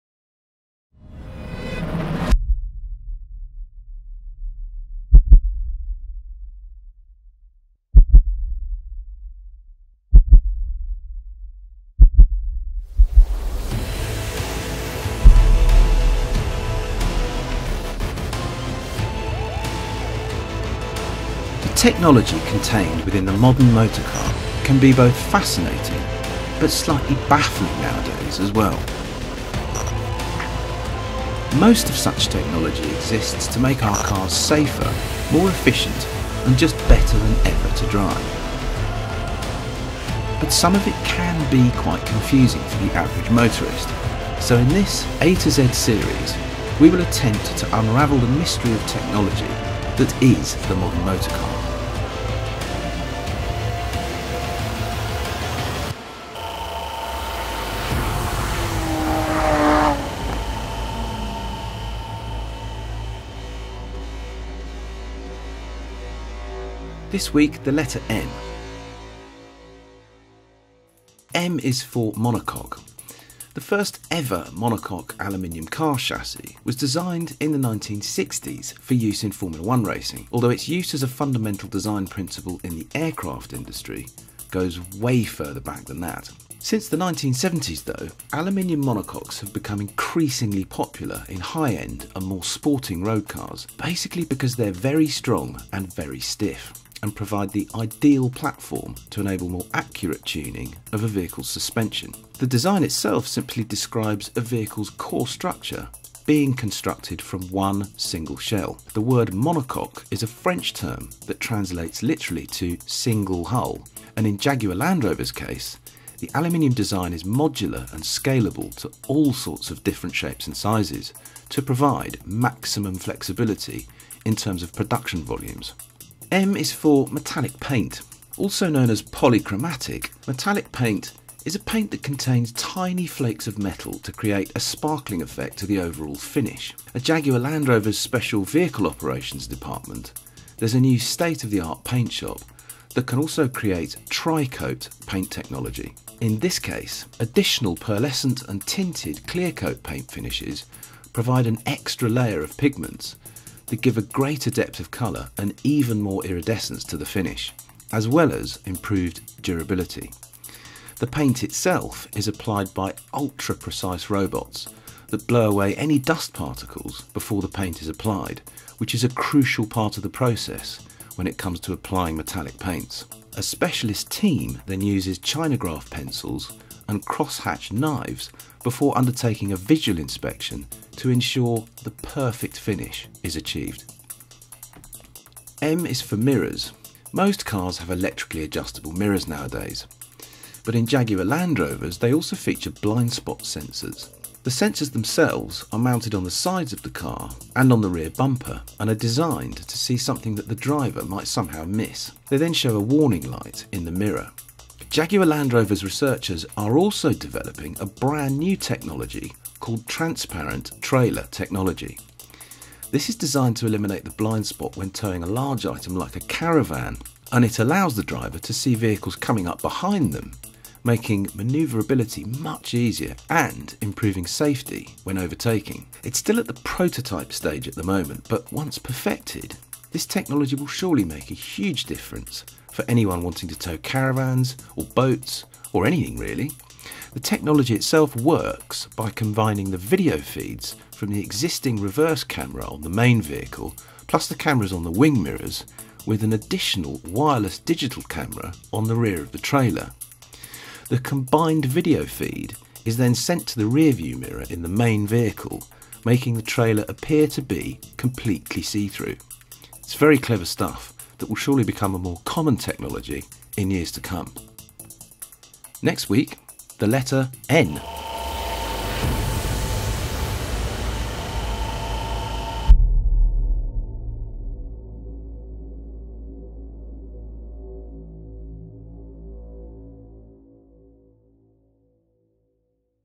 Car Tech A to Z: M is for monocoques, metallic paint and mirrors...
The technology contained within the modern motor car can be both fascinating and slightly baffling. Most such technology exists to make our cars safer, more efficient and just better than ever to drive but some of it can be quite confusing for the average motorist. In this A to Z series we will attempt to unravel the mystery of technology that is the modern motor car. This week, we’re visiting the letter M…